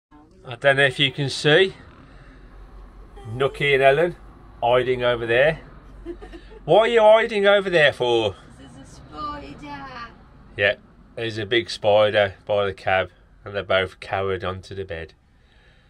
HUSKY SCARED!! HELEN & NANOOK ARE HIDING!
Helen and Nanook are scared.

They're hiding from a spider! What are they like!